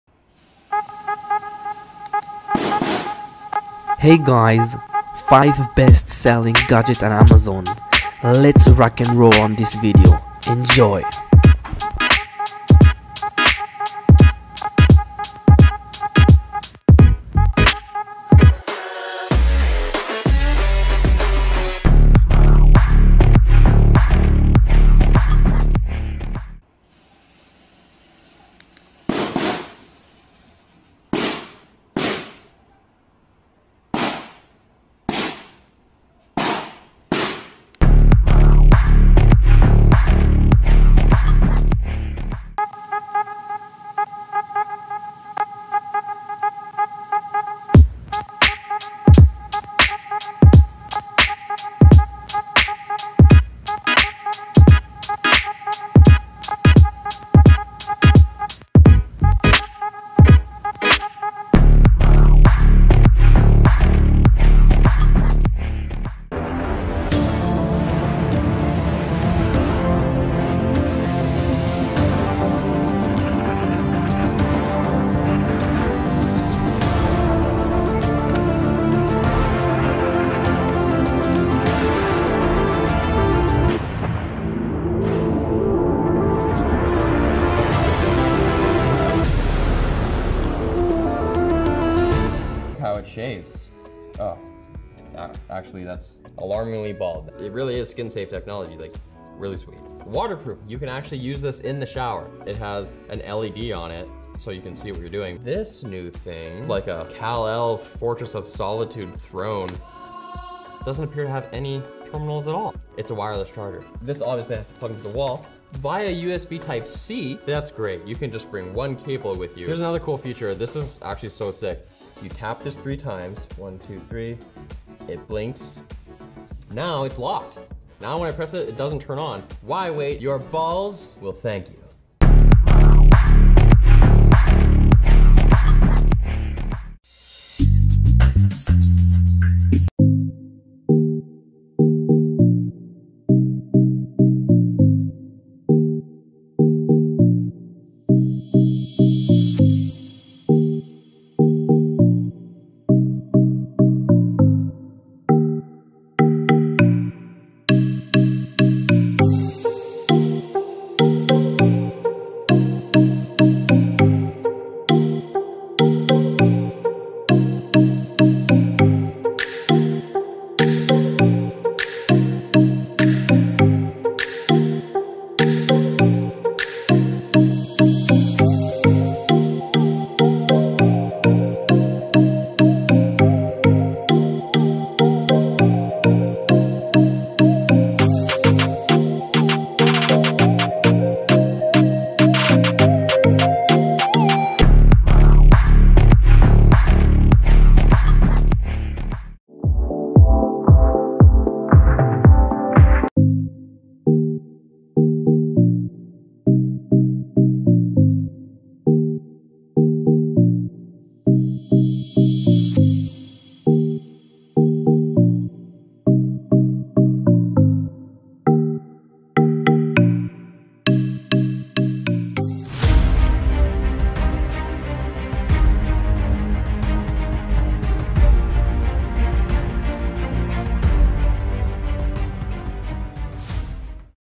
5 BEST Selling Amazon Gadgets 2022 - YOU MAY NEVER KNEW EXIST - enjoy #4
5 BEST Selling Amazon Gadgets - YOU MAY NEVER KNEW EXIST - #3
In this video I will show another selection of best selling gadgets 2022 from Amazon. Amazing Products. Item. Best Tech. Smart Gadgets. Review Amazon Goods. Review China Gadgets. ↓↓LINKS TO PRODUCTS BELOW↓

1. JCKSY Blue Beam Pointer, Long Range High Power Burning
2. Xprite LED Red Blue Windshield Dash Emergency Strobe Lights
3. MANSCAPED™ Electric Groin Hair Trimmer
4. Travel Laptop Backpack Water Resistant
5. Hexagonal Wall Light Modular Touch Sensitive Lights